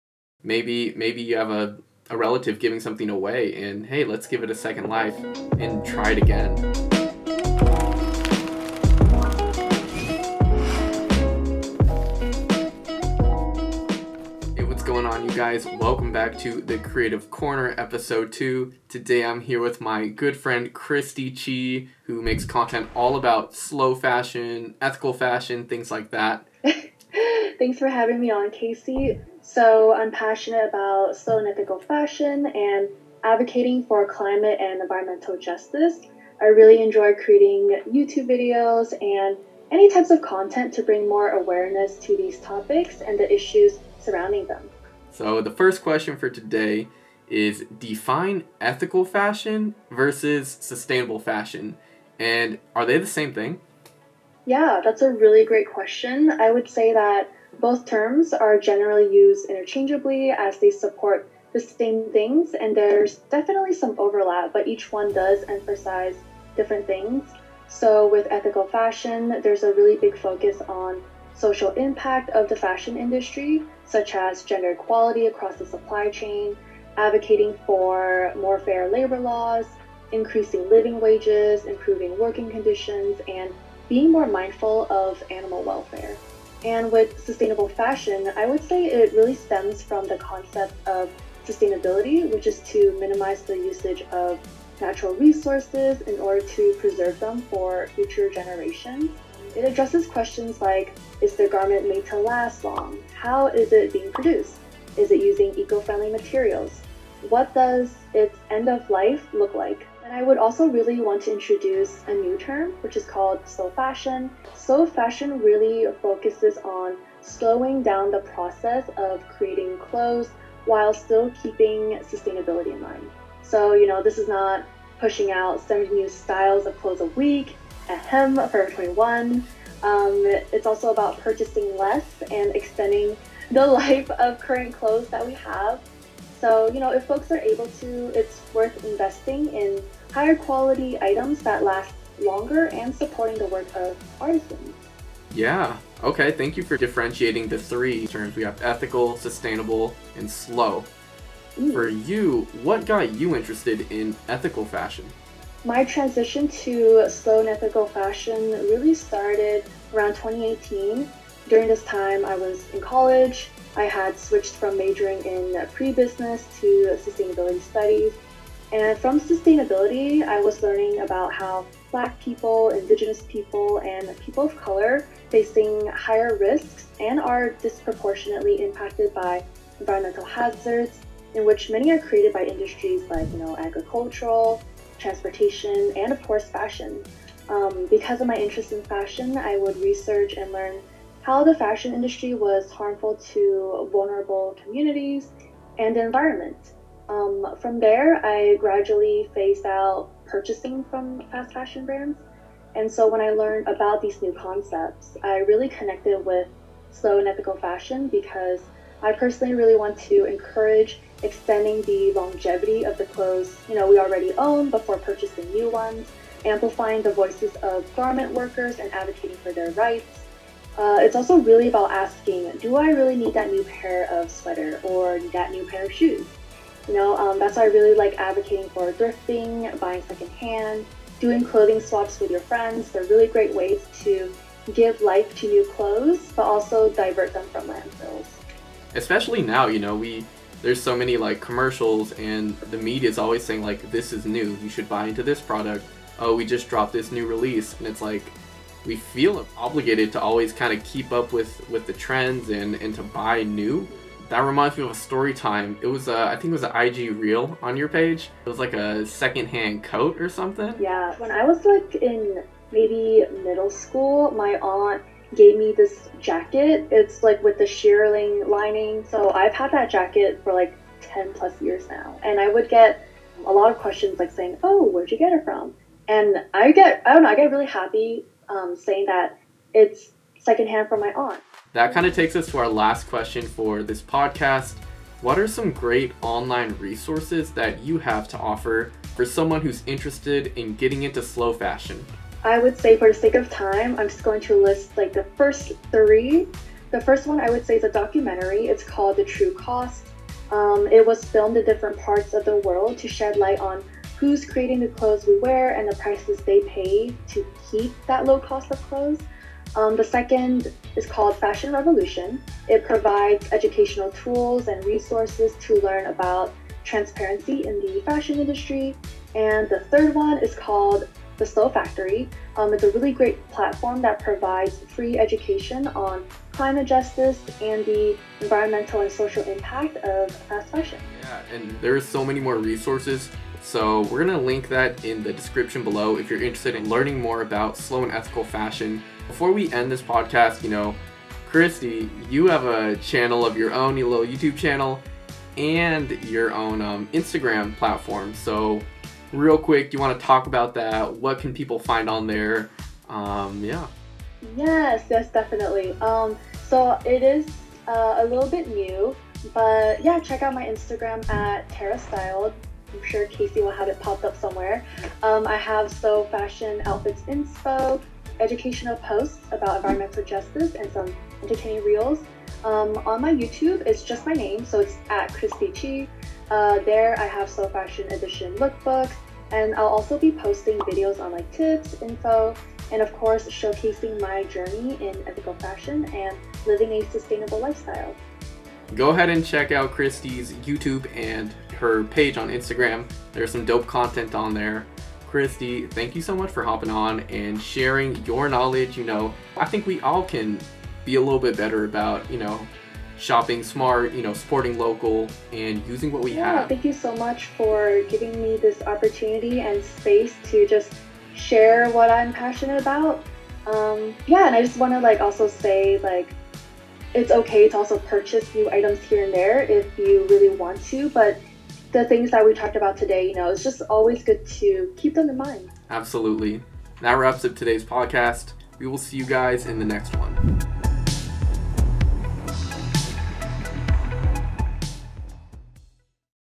Let's Talk about Slow Fashion | Ep. #2 - The Creative Corner
Shopping sustainably is more than a "2021 "trend. In this episode, we break down the idea of slow fashion vs. fast, shopping sustainably, and the impact of the fashion industry on the world! it's a lot deeper than a Logo.

Slow Fashion Resources:
1. The True Cost - documentary
Filmed in different parts of the world to shed light on who’s creating the clothes we wear and the prices they pay to keep the low cost of clothes

2. Fashion Revolution
Provides educational tools and resources to learn more about transparency in the fashion industry

3. The Slow Factory
Platform providing free education on climate justice, and the environmental and social impact of fast fashion

4. Good on You
Platform with articles, guides, and a rating system that rates how sustainable each fashion brand is
A growing list of sustainable brands to support

5. Clean Clothes Campaign
Website with articles, published papers, and resources to learn more about worker’s rights and how to support garment workers around the world

6. Intersectional Environmentalist →  On website, Fashion Section
Great place to discover orgs and a diverse range of experts and other advocates knowledgeable about sustainable fashion

Video Produced by Kasey Bruce Media - San Francisco Video Production Company.